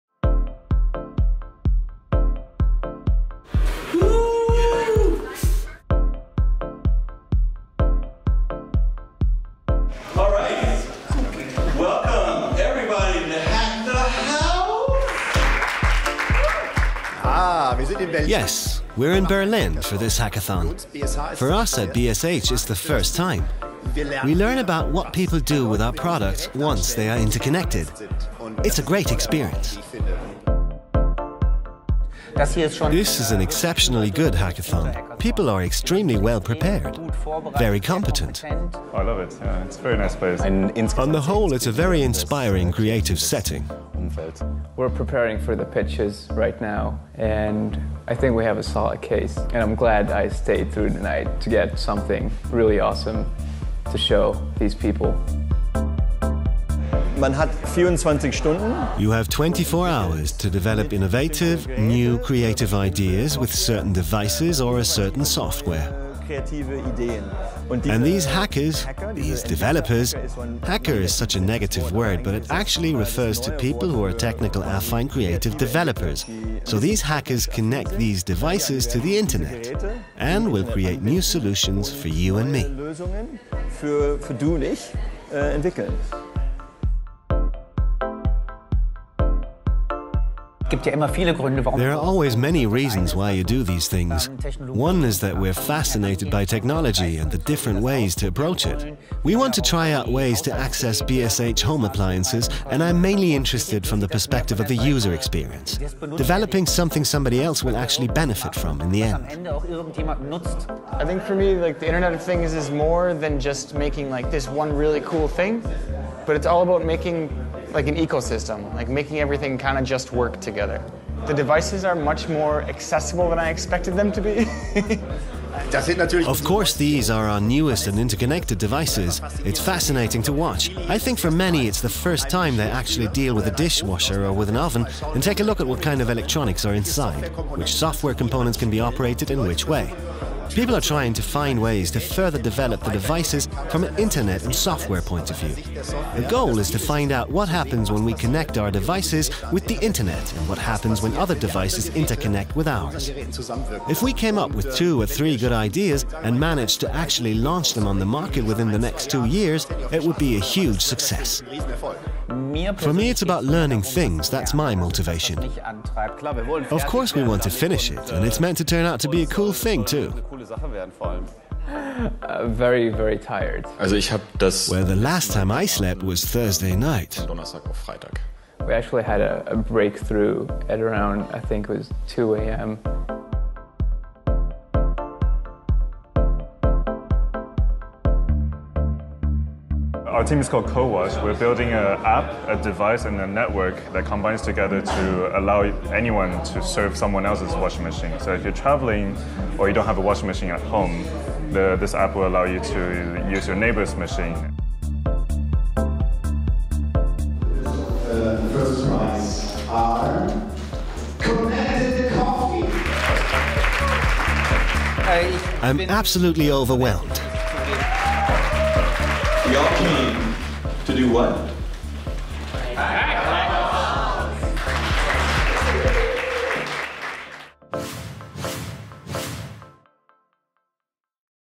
First BSH-Hackathon
In response to an invitation by BSH and in cooperation with relayr around 40 engineers came together for three days to develop new ideas for networking home appliances.